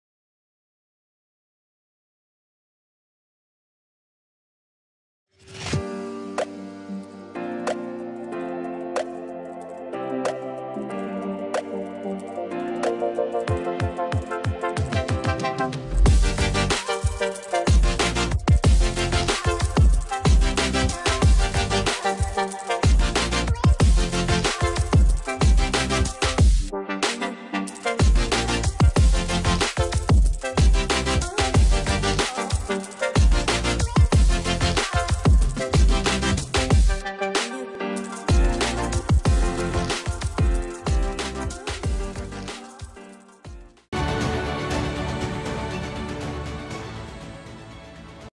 Giant “3D” Pandas Appear in LianTronics' Video Wall in Chengdu
HELLO, PANDAS! Huge 3Dpandas appears in the L-shaped LianTronics LEDwall in Chengdu, saying hello to the public!